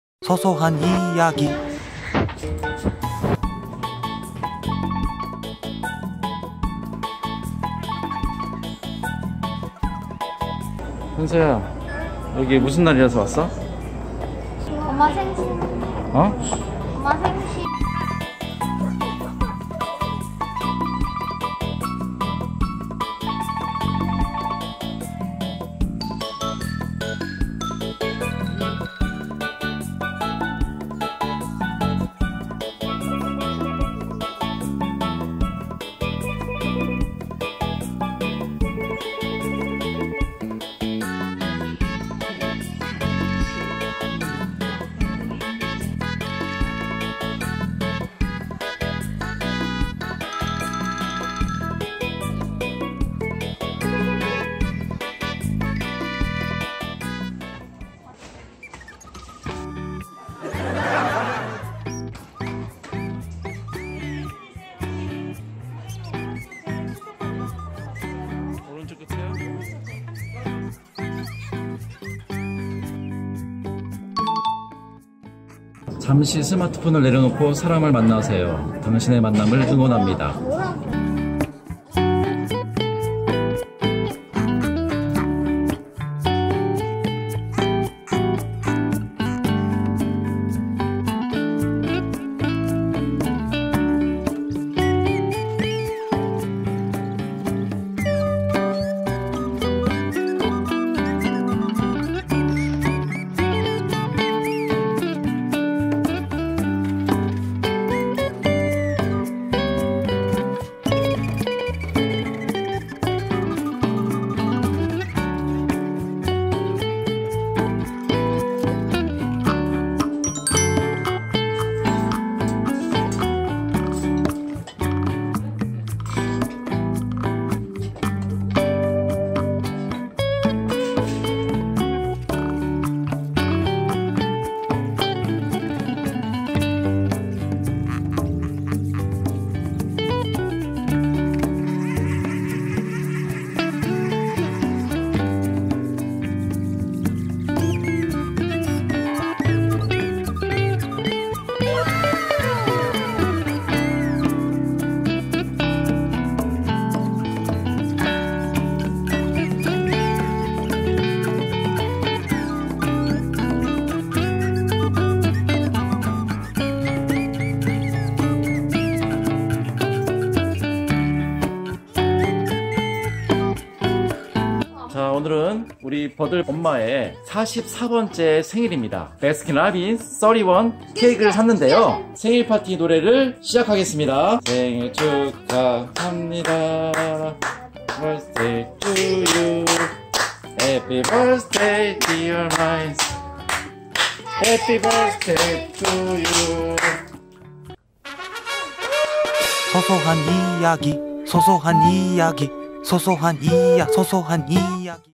생일 식사 / 강강술래 [소이]
질문은 댓글에~!^^

사랑하는 아내가 44번째 생일을 맞이한날
아이들과 식사하고 아이스크림 케잌도 먹었던 즐거운 날이였어요